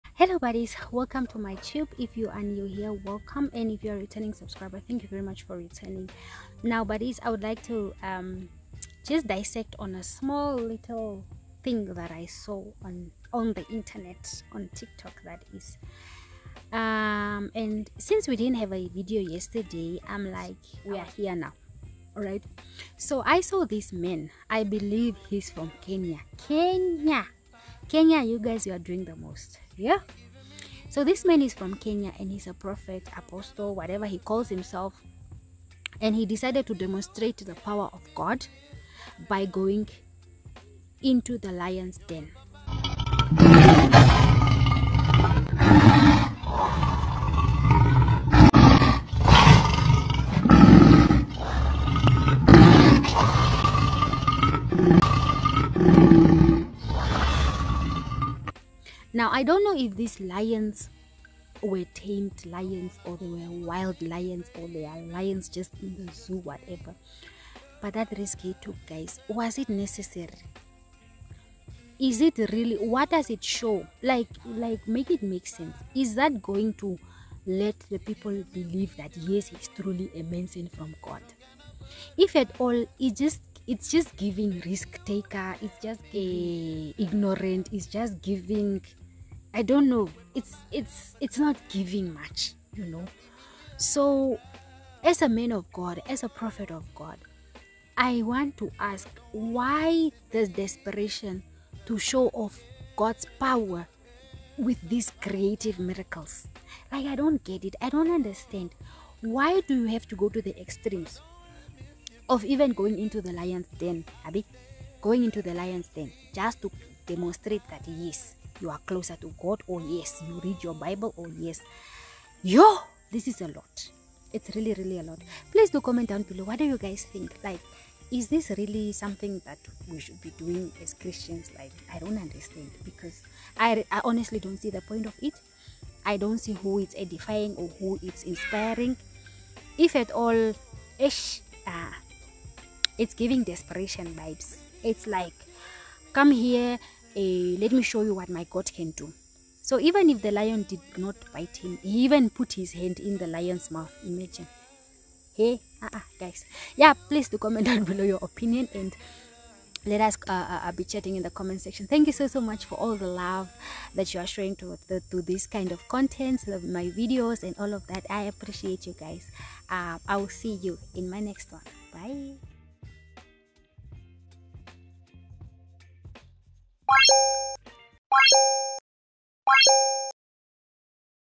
PASTOR LOCKS HIMSELF IN THE LIONS DEN TO DEMONSTRATE THE POWER OF GOD ||WHY MAARA?
a man from Kenya shows the power of God...is this necessary??
Prophet Nana Poku is not here to play.. but is this right